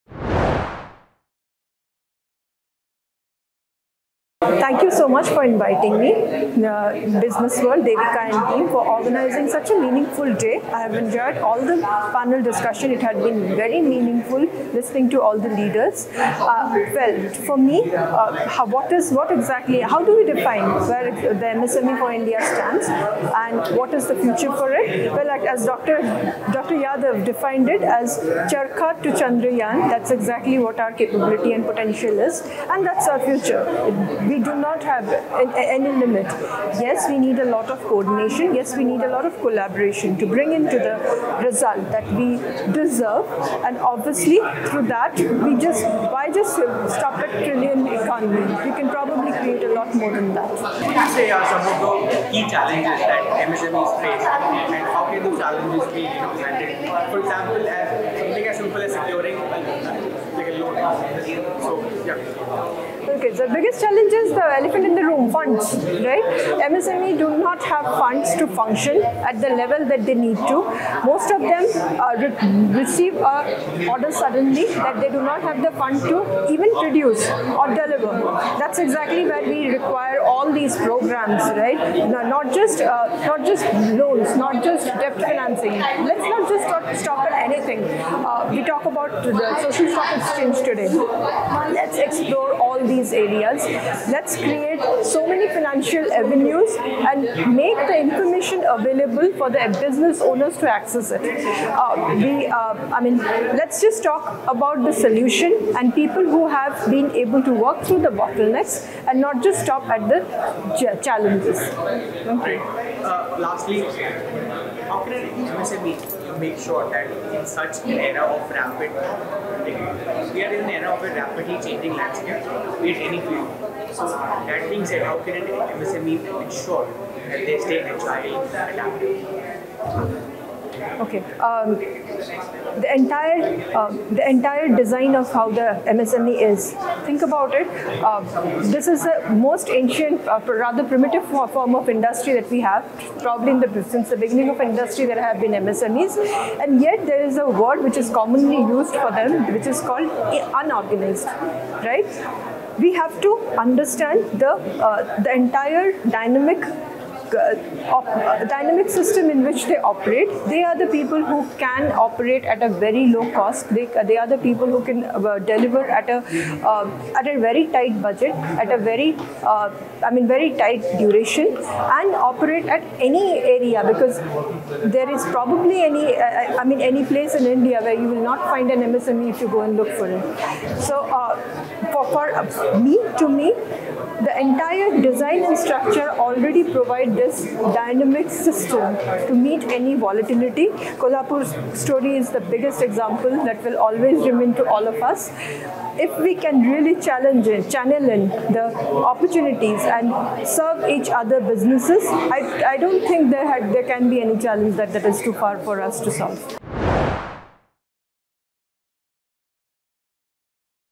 Nabomita Mazumbdar, President of India Awardee, Founder - Nabomita.com &National Chairperson - CIMSM
BW Emerging Business Summit - 24th Aug 2023, Park, New Delhi